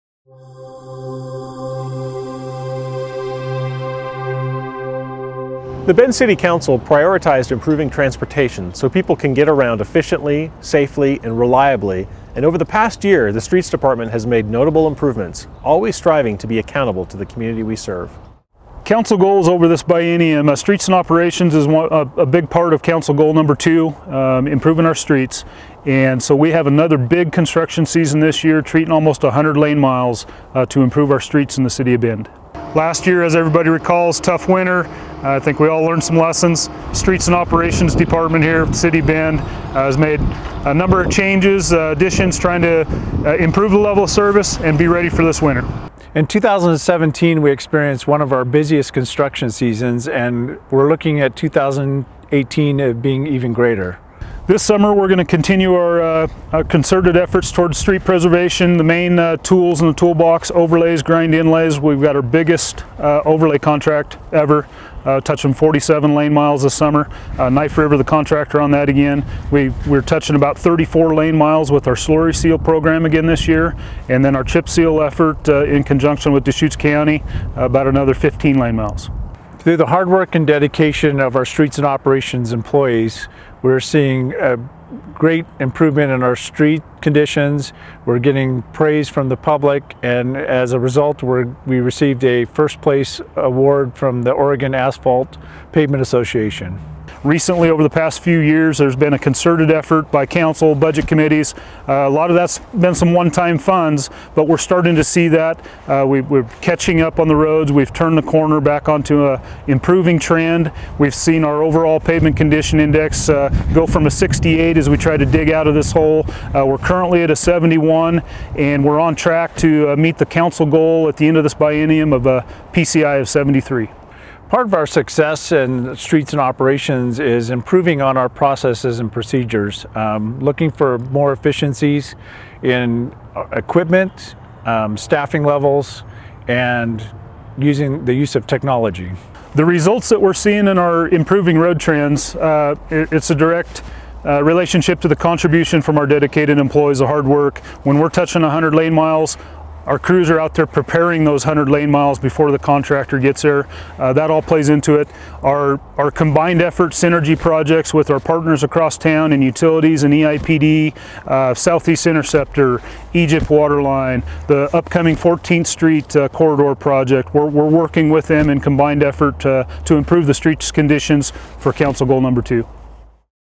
City Council Goal 2 - Street Preservation
The Bend City Council prioritized improving transportation so people can get around efficiently, safely and reliably and over the past year, the Streets Department has made notable improvements, always striving to be accountable to the community we serve.